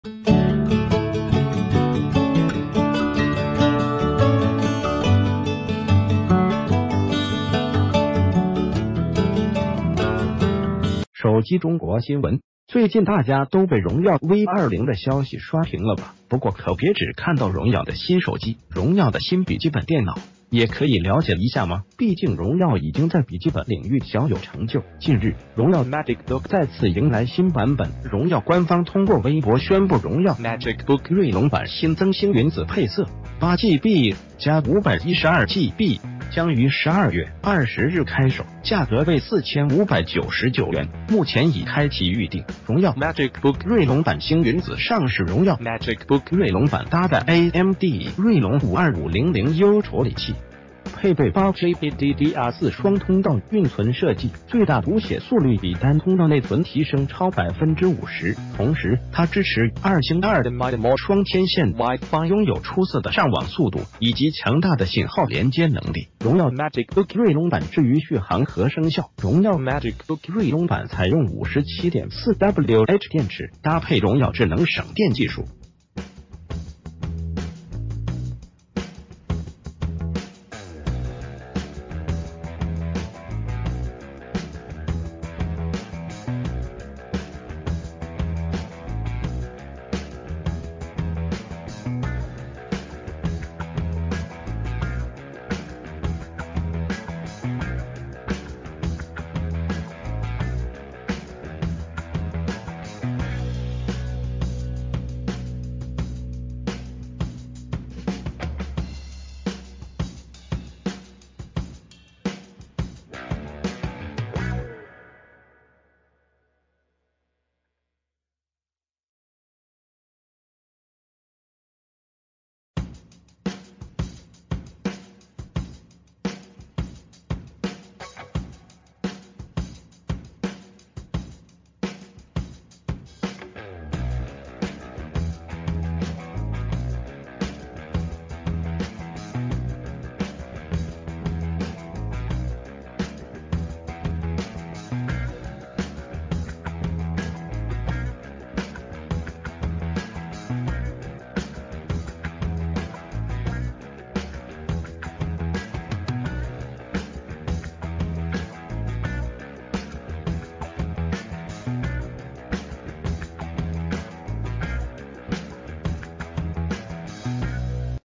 榮耀官宣唯美新品 8GB+512GB版僅4599
谢谢！

    【手機中國新聞】最近大家都被榮耀V20的消息刷屏了吧，不過可別只看到榮耀的新手機，榮耀的新筆記本電腦也可以了解一下嘛，畢竟榮耀已經在筆記本領域小有成就。近日，榮耀MagicBook再次迎來新版本。榮耀官方通過微博宣布，榮耀MagicBook銳龍版新增星云紫配色（8GB+512GB），將于12月20日開售，價格為4599元，目前已開啟預定。
    榮耀MagicBook銳龍版星云紫上市
    榮耀MagicBook銳龍版搭載AMD銳龍5 2500U處理器，配備8GB DDR4雙通道運存設計，最大讀寫速率比單通道內存提升超50%。同時，它支持2×2 MIMO雙天線WiFi，擁有出色的上網速度以及強大的信號連接能力。
    榮耀MagicBook銳龍版
    至于續航和聲效，榮耀MagicBook銳龍版采用57.4Wh電池，搭配榮耀智能省電技術，平時使用時續航提升15%。而在聲效上，榮耀MagicBook擁有由杜比調教的四個揚聲器，雙高音直達耳朵，雙低音更加渾厚，在杜比全景聲的加持下，讓你的耳朵被美妙的聲音纏繞。
    星云紫配色比較適合女性用戶使用，感興趣的朋友可以關注一下。